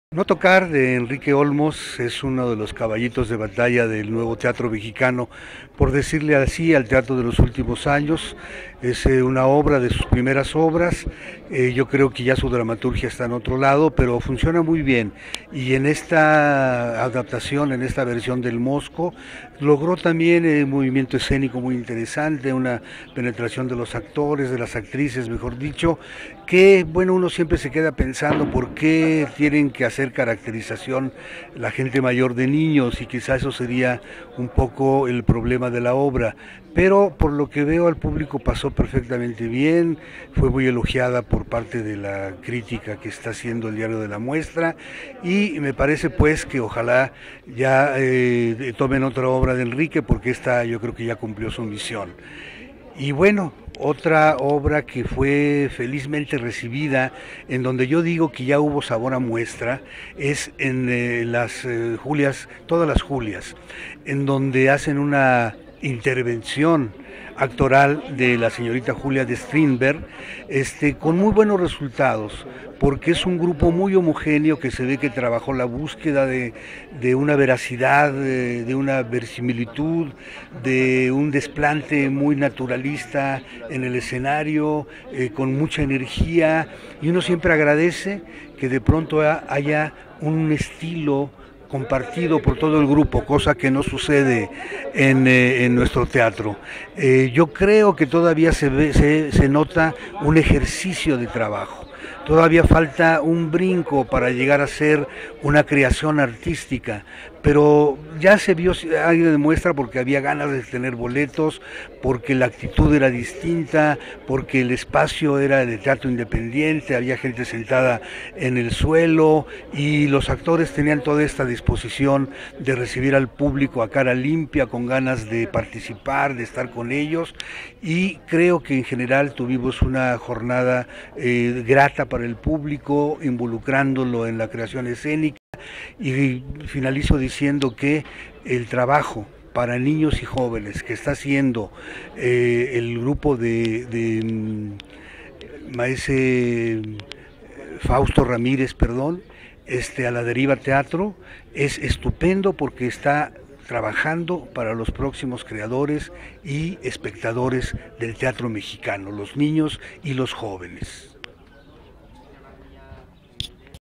La quinta mirada. No tocar y Todas las Julias del mundo
Comentario de Fernando de Ita sobre las obras No tocar de Enrique Olmos dirigida por Luis Manuel Aguilar Mosco y Todas las Julias del mundo  de A la deriva teatro en la 33 MNT (SLP 2012)